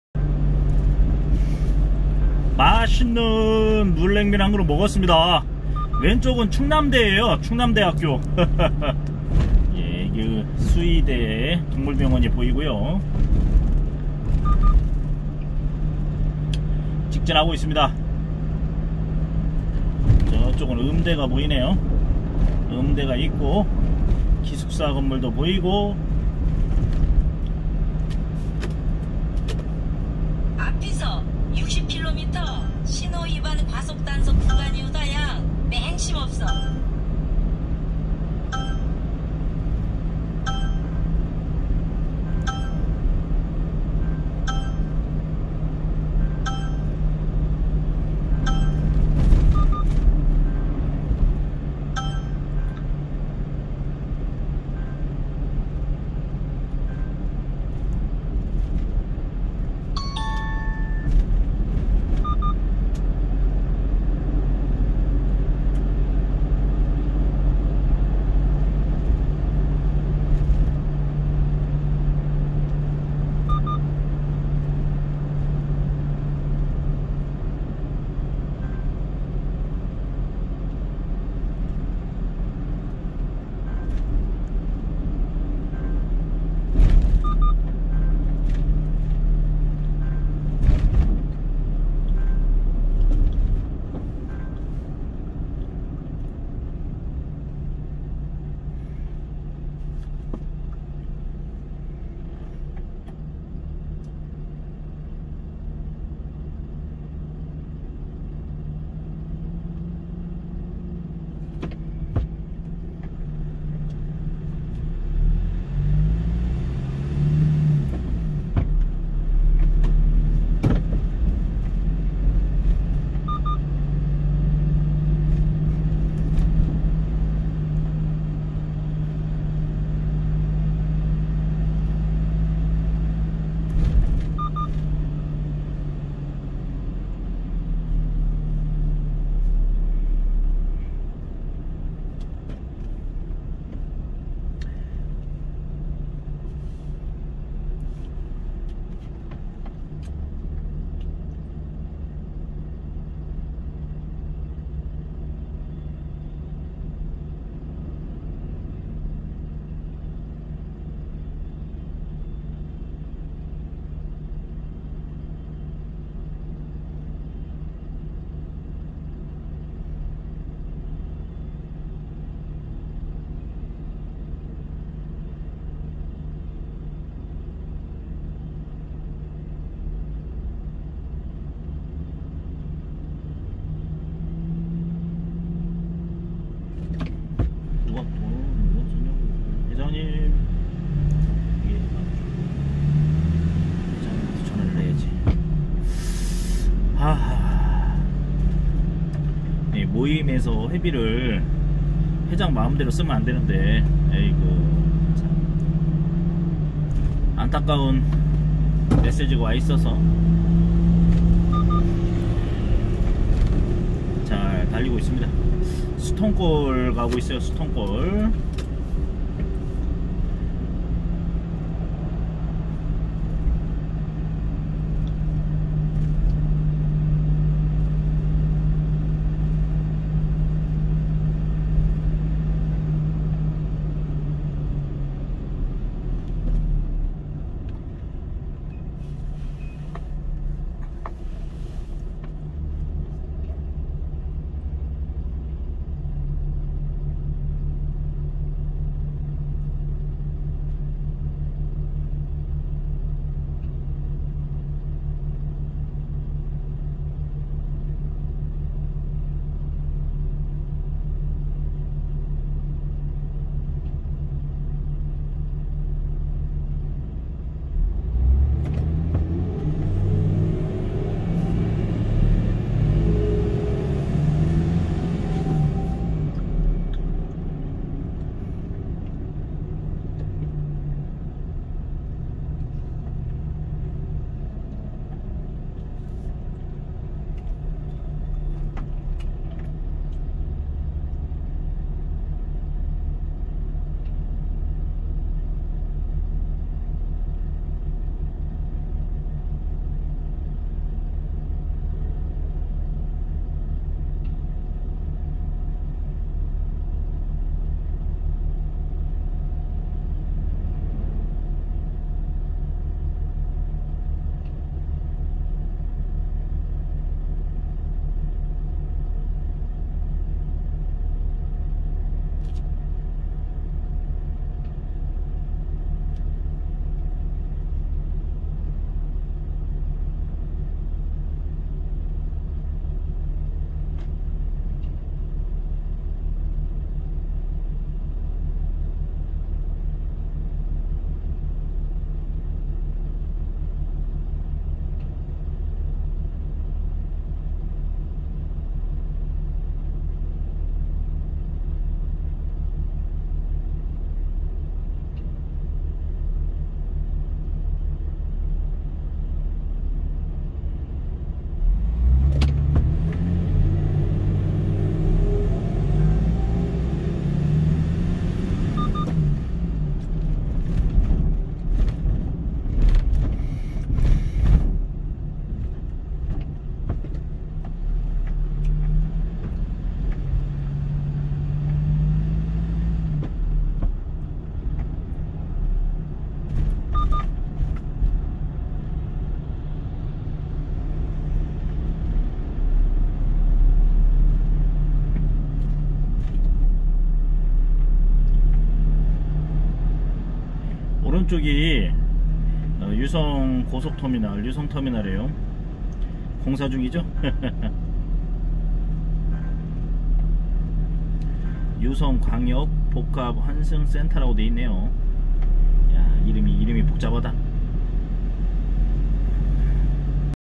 유성구 신성동 자운대에서 구암동 구암역까지 1톤 트럭커 제주흑돼지 싣고 달려요 feat 고기왕 제주아재
유성구 신성동 자운대에서 충남대학교지나서 구암동 구암역까지 1톤 트럭커 제주흑돼지 싣고 달려요 feat 고기왕 제주아재